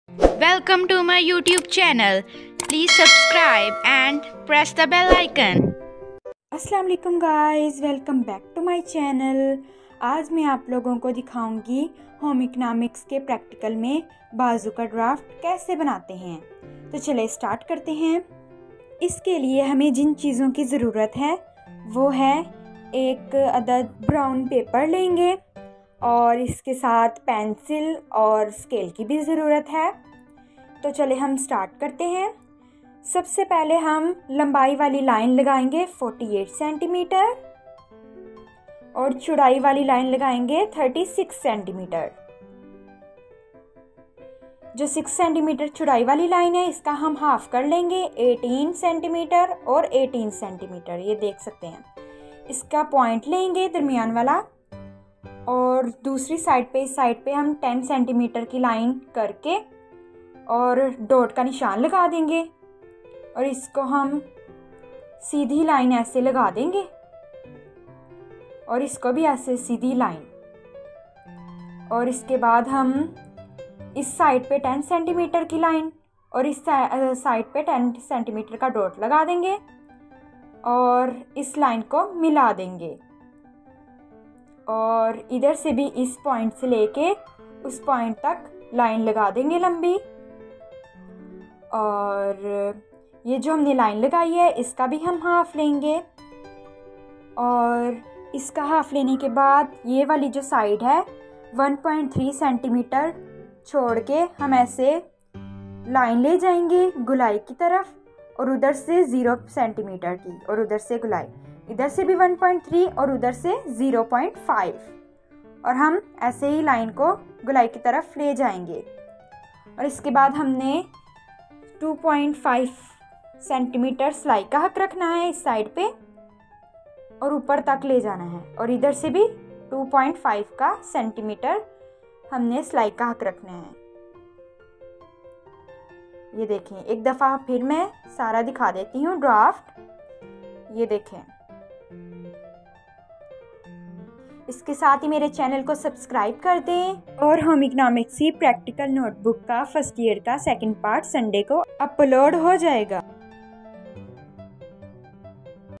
home economics ke practical main bazo ka draft
This video is for 2nd year students who finds difficult to make practical copy for their practical exam. Here on my channel you can also see practical copy of all subjects including physical education, home economics, psychology, computer etc. How to make bazo draft watch This video is for 2nd year students.